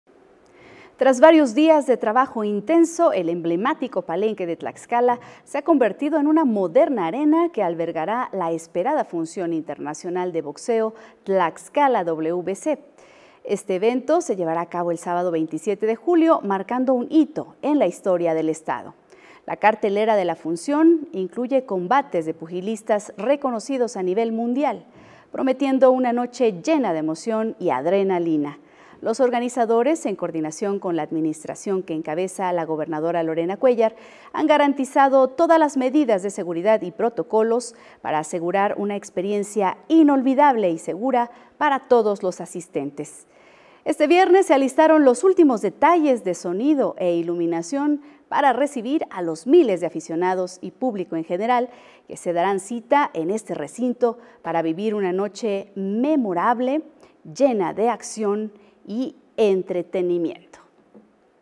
Todo listo para la gran Función Internacional de Box Tlaxcala WBC 2024
El Palenque de Tlaxcala se encuentra listo para  recibir a los miles de aficionados que esperan vivir una noche memorable llena de acción y entretenimiento.